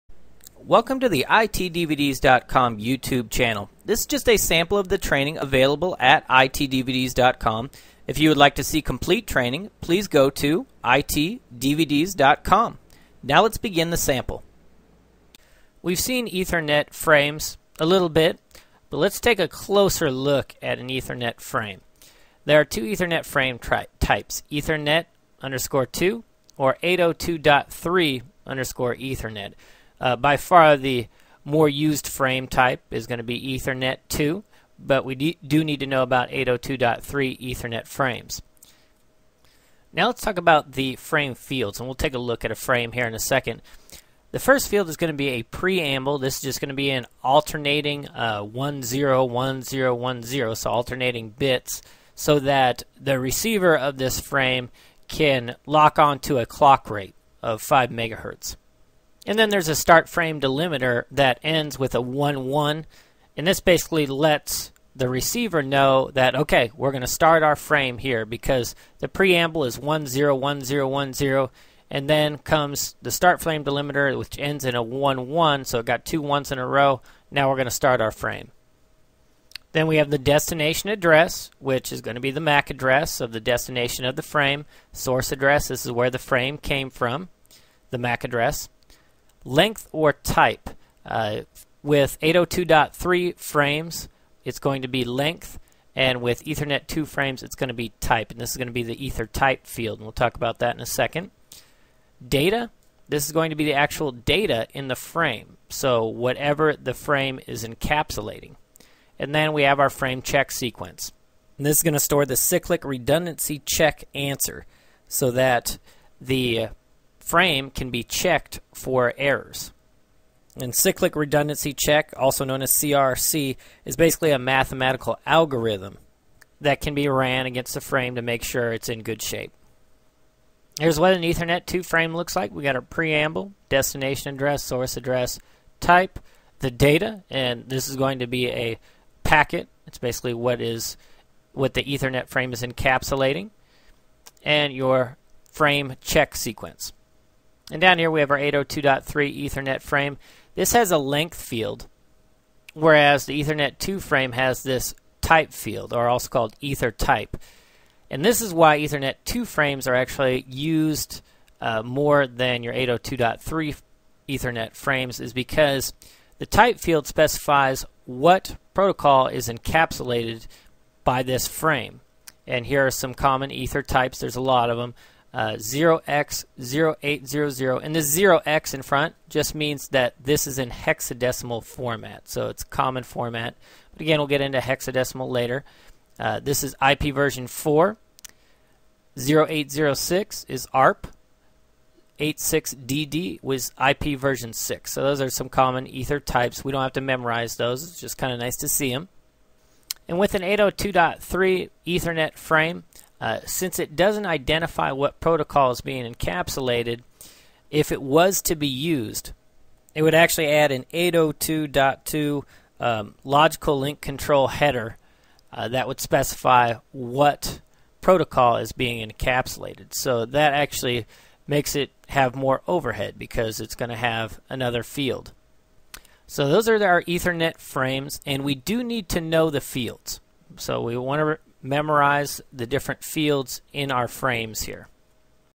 CCNA Training CBT - Ethernet Frames
Cisco Certified Network Associate CCNA training by ITDVDS.com to prepare for Exam 640-802.  This is just a sample from ITDVDS.com. In this sample we go over Ethernet Frames.